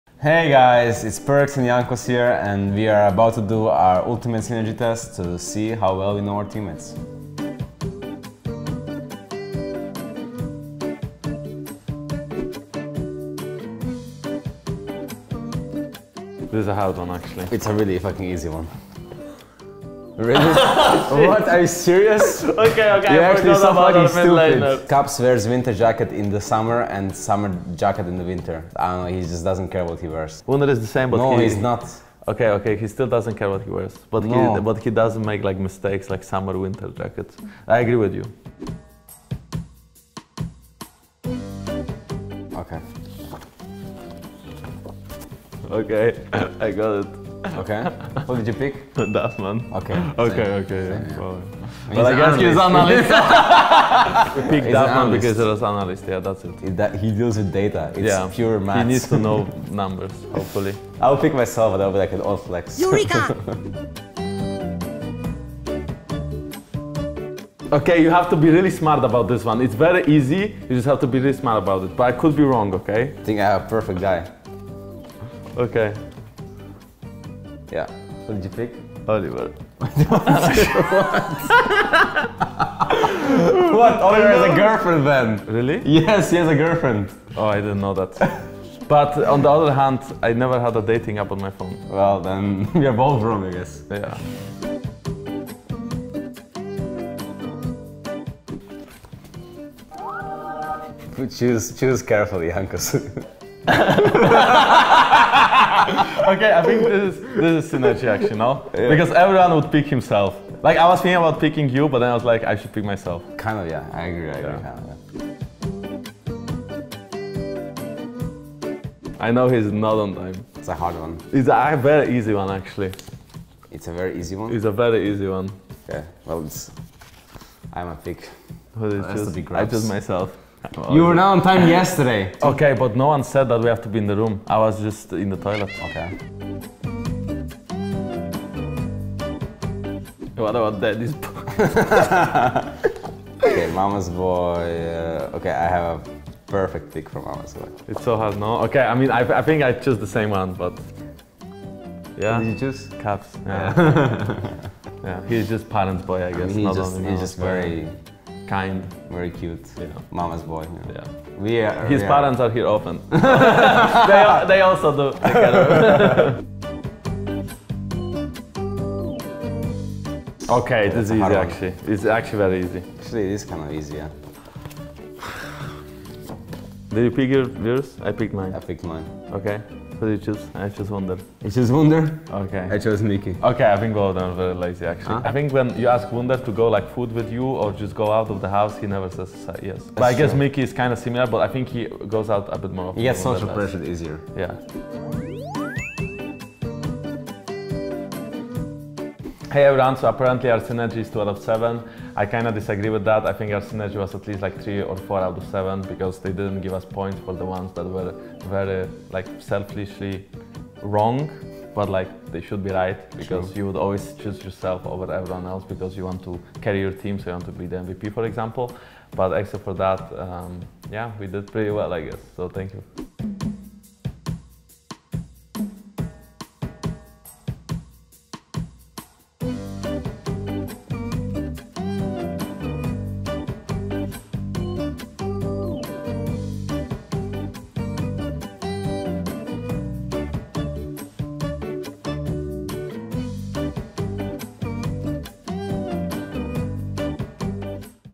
Ultimate Synergy With PERKZ and Jankos | G2 League Of Legends
PERKZ spent a year in the botlane, but did it affect his mid / jungle synergy with Jankos? Find out in this episode of Ultimate Synergy!